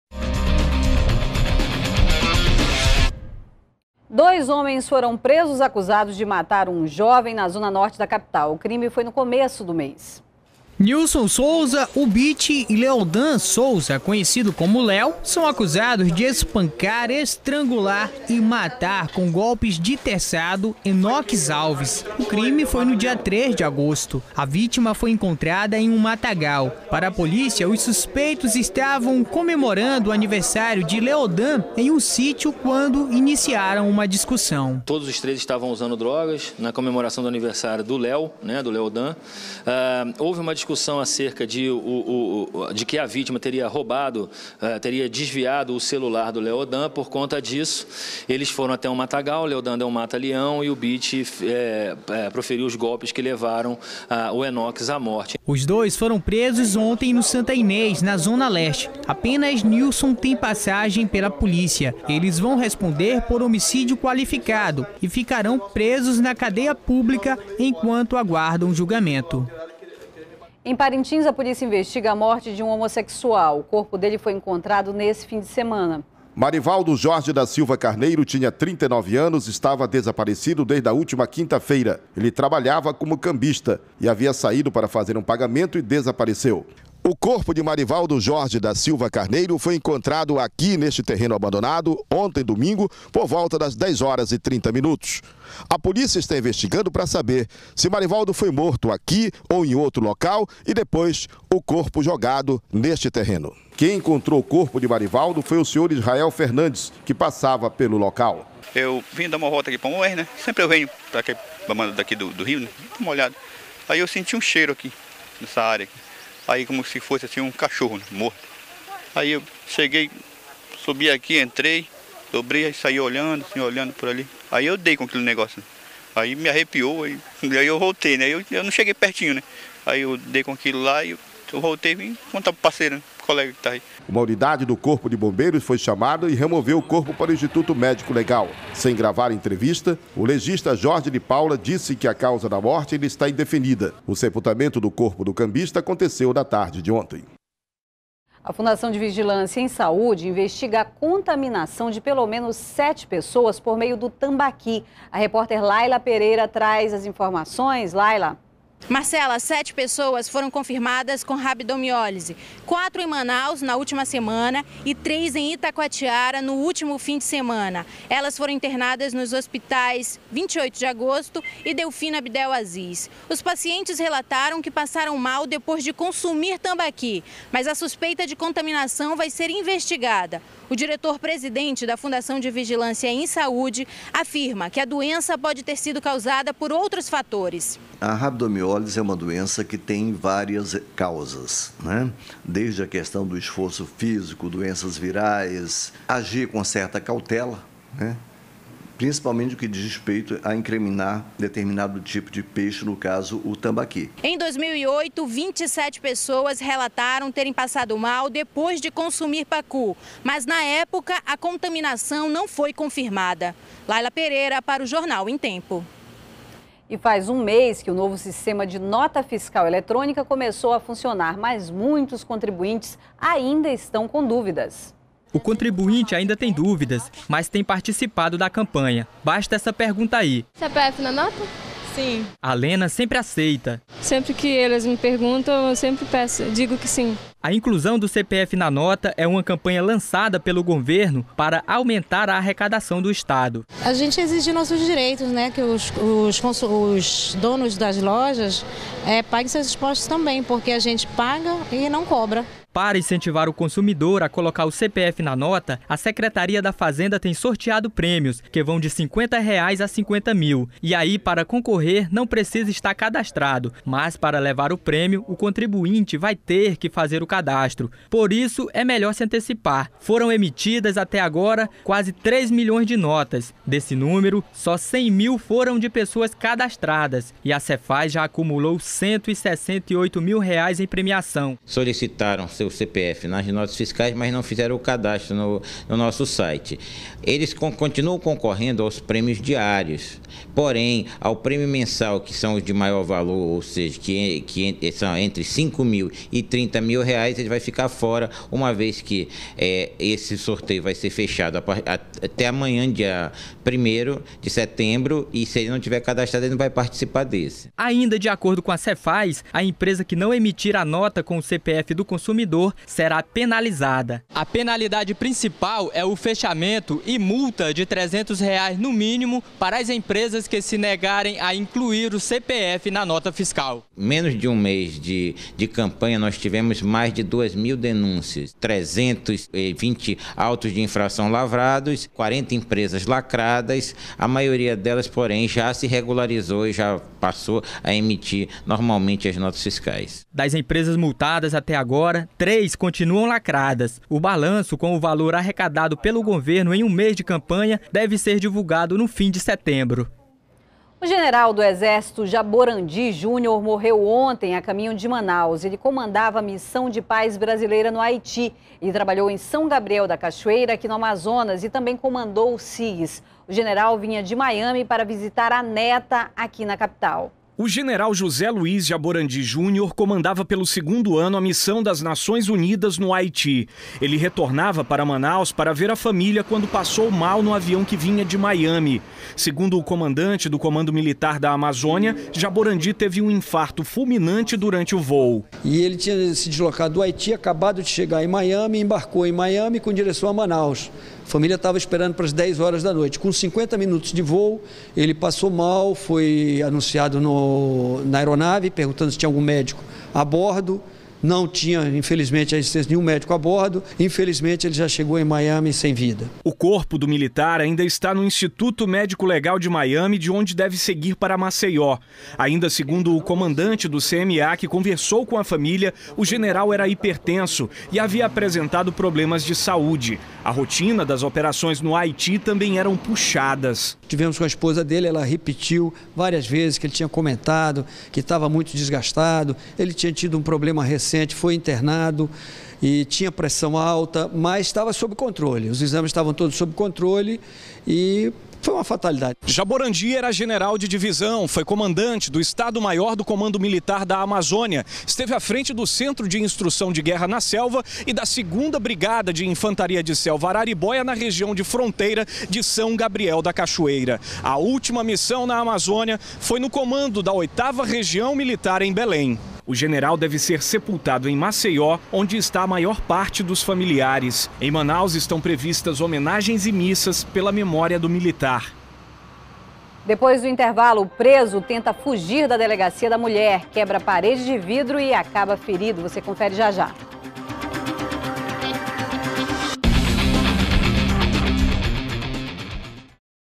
General do Exército infarta e morre a caminho de Manaus
02º BLOCO JORNAL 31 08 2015 - General do Exército infarta e morre a caminho de Manaus